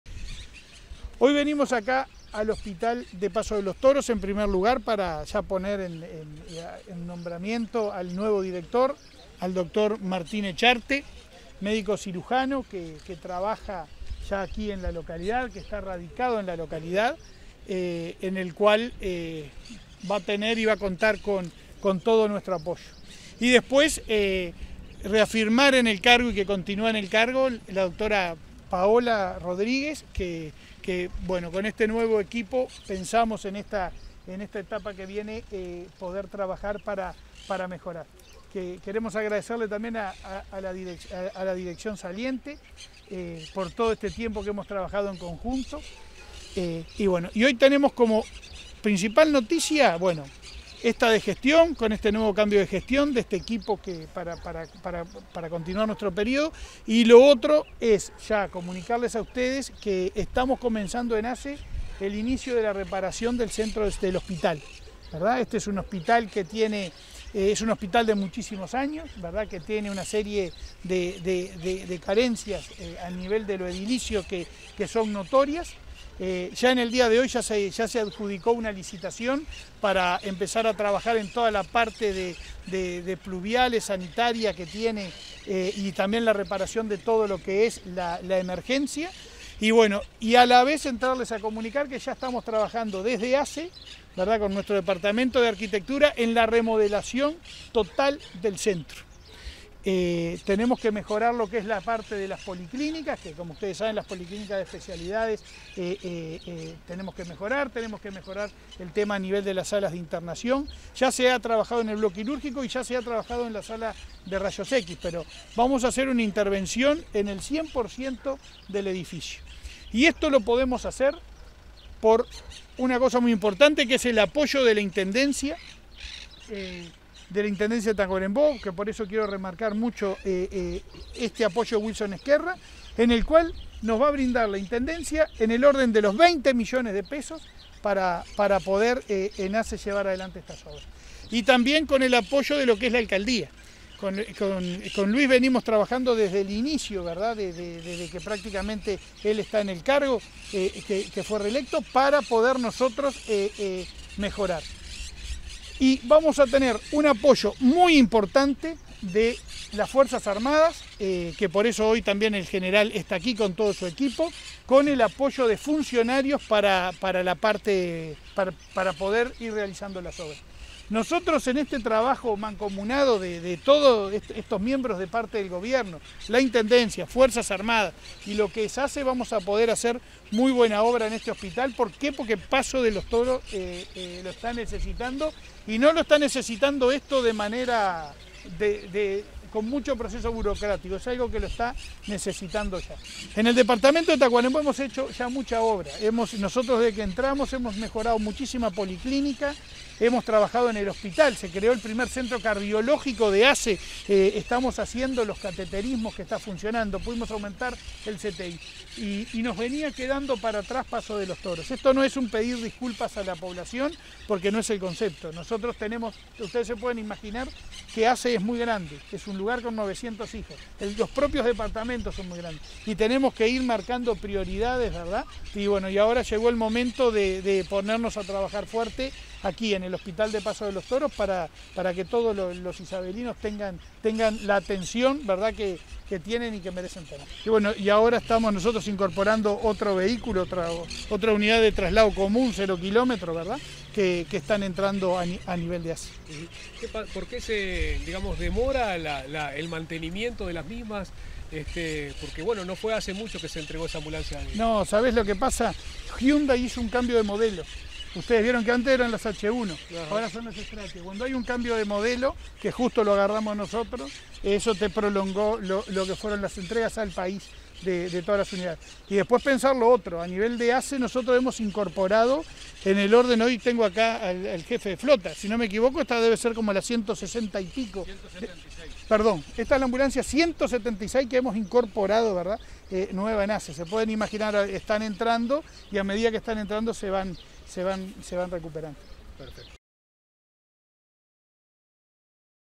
Hospital de Paso de Toros recibió ambulancia 0KM y se anunció remodelación edilicia completa
Hospital de Paso de Toros recibió ambulancia 0KM y se anunció remodelación edilicia completa del Centro Asistencial
Este martes 17 de octubre, el Presidente de ASSE, Leonardo Cipriani, visitó el Hospital de Paso de los Toros, para participar de la toma de posesión de cargo del nuevo Director del centro asistencial, el Dr. Martín Echarte. En la oportunidad, se entregó una ambulancia 0 kilómetro para completar la dotación de unidades de traslado, y se anunció la realización de una remodelación edilicia completa con el aporte de la Intendencia de Tacuarembó.

En ese sentido, el Intendente Wilson Ezquerra, informó que el gobierno departamental colaborará con 500 mil dólares en materiales para la mejora de la infraestructura del Hospital.

Por su parte, Leonardo Cipriani agradeció la colaboración permanente de la Intendencia, y anunció que en el día de hoy se adjudicó una licitación los arreglos de la red pluvial y la sala de emergencias, mientras que desde arquitectura del organismo ya se vienen proyectando la remodelación total del Hospital, ya que se requieren "intervenciones profundas" y serán del "100%. El jerarca indicó que se contará también con el apoyo de mano de obra de las Fuerzas Armadas.

Recordó que junto al MTOP, ya se adecúo la caminería y se intervino el predio del fondo del nosocomio.
Tanto Ezquerra como Cipriani, valoraron el trabajo mancomunado que se viene llevando adelante junto al alcalde Luis Irigoín desde el inicio de la gestión, que ha posibilitado la realización de distintas obras en Policlínicas pertenecientes de la Red de Atención Primaria de Tacuarembó.

En este momento, se viene trabajando para la concreción del Primer Centro Departamental de Adicciones, con una inversión de un millón de dólares de la Intendencia y los recursos humanos de ASSE.

FORTALECIMIENTO DEL CONVENIO DE COMPLEMENTACIÓN PARA USUARIOS DE SANIDAD MILITAR

Por otro lado, y con la participación del Director de Sanidad Militar, Hugo Rebollo, ASSE fortaleció el convenio de complementación existente entre ambas instituciones.
De esta manera, se eliminan trabas burocráticas y los usuarios de Sanidad Militar podrán concurrir a los centros asistenciales de ASSE solo con su cédula de identidad.
Esto mejora la accesibilidad a una atención oportuna, de calidad e integral, de la misma forma que la reciben los usuarios del prestador público.